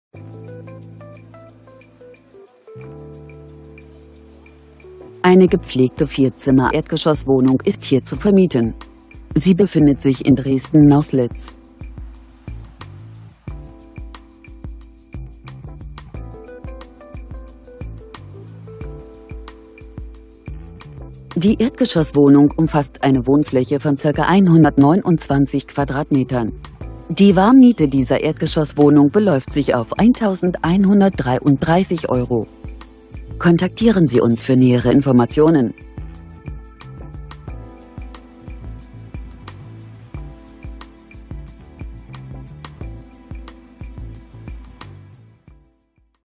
4 Zimmer in Naußlitz - neu saniert!
Ergeschosswohnung
Zimmer: 4
Wohnfläche: ca. 129,46m²

Andreas Pietschmann

Westringstraße
04435 Schkeuditz, OT Dölzig